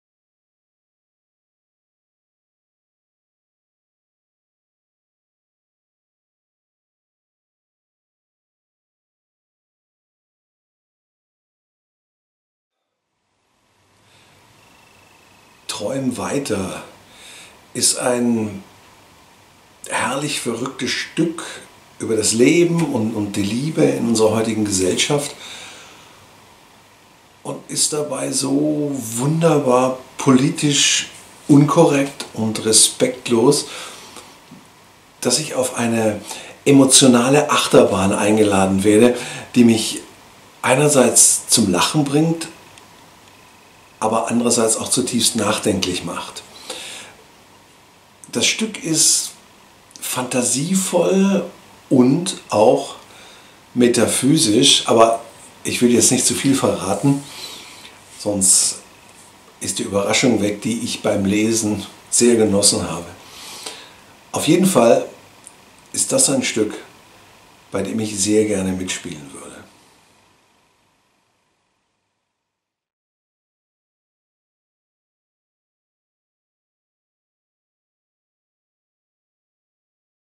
Träum weiter von Nesrin Şamdereli // O.- eine Stadt sucht ein Drama, Thomas Birklein
O.- EINE STADT SUCHT EIN DRAMA!

Träum weiter von Nesrin Şamdereli
Lesung am 29.09. um 15 Uhr im Spielraum